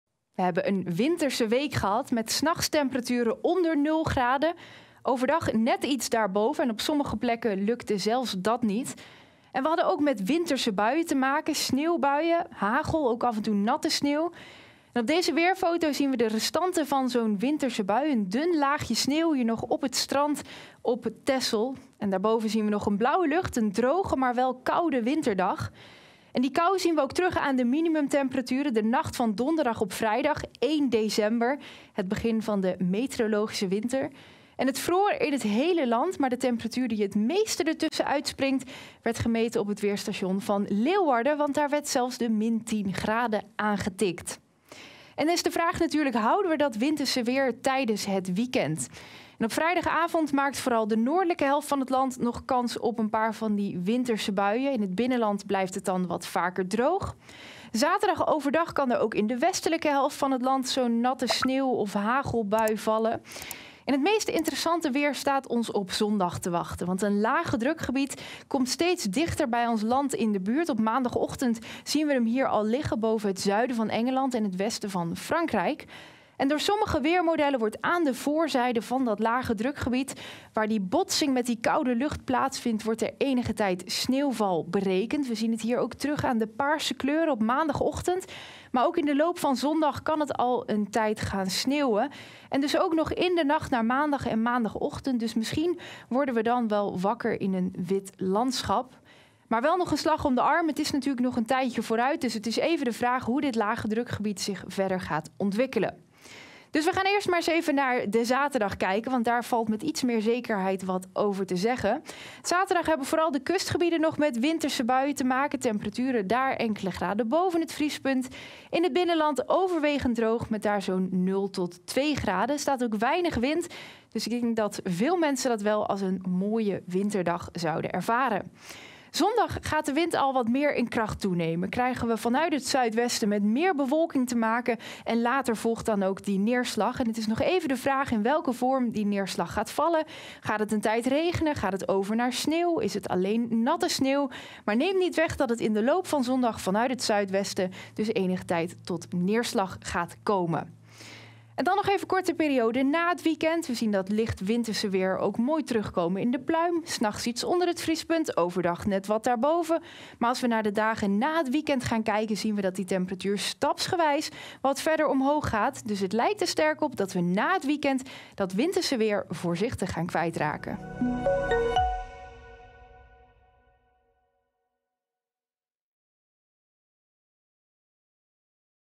Weekend weerbericht: gaat het weer sneeuwen? ☃️
Vallen er dit weekend nog een paar winterse buien of gaat het langere tijd sneeuwen? Roosmarijn praat je bij met de weersverwachting voor het eerste weekend van december.